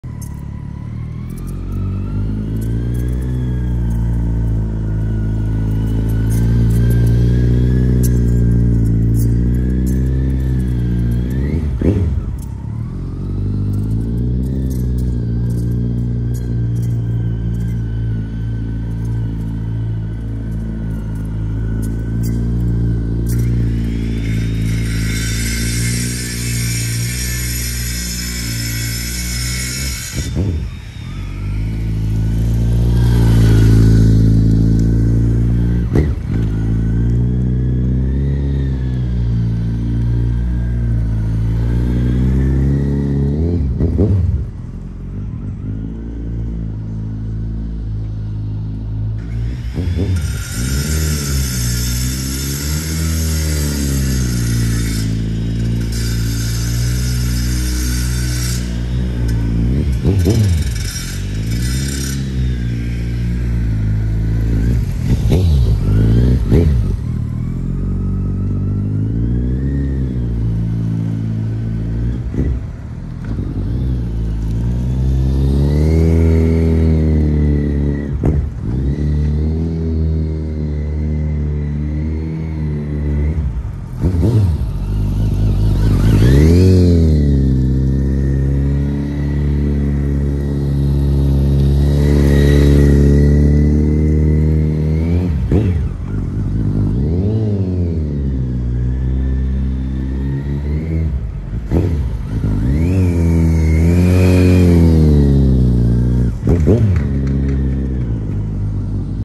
Kawasaki Concours 14 (GTR 1400 / ZG1400C) Slow riding U-turn practice
Doing some slow riding practice, figure 8, handlebar lock to lock transitions.  I prefer going out riding, however, I have three young ones at home and couldn’t leave them by themselves. This video might be boring for some, but it shows what this motorbike can do at slow speeds. This motorbike is also used by many police agencies across the United States.